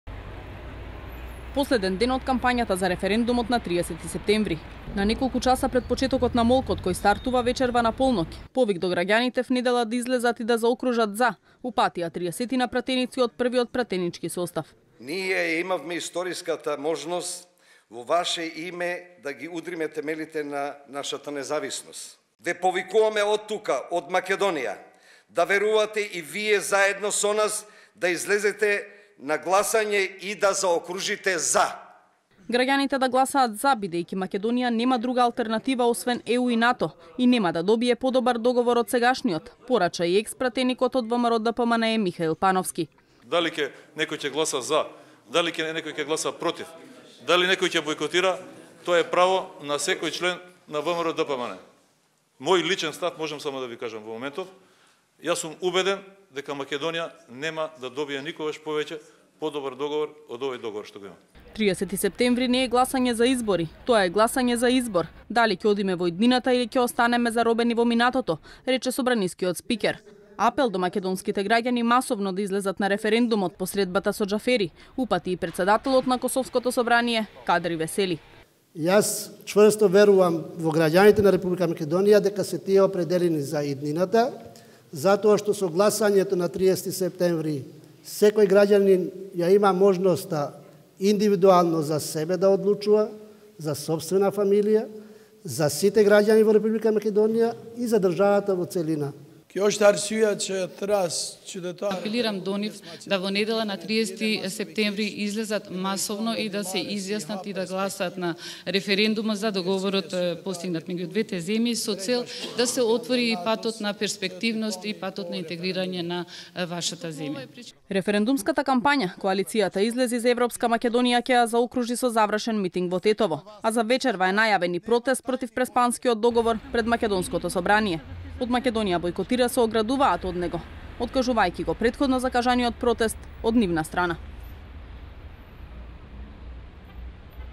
Првите македонски пратеници ќе гласаат „ЗА” на референдумот
Ако ја сакате Македонија ќе излезете на референдумот и ќе гласате. Ова го порачаа пратениците од првиот парламентарен состав. И тие слично на Владата се на став дека  Договорот со Грција не го загрозува идентитетот, туку дека го зацврстува.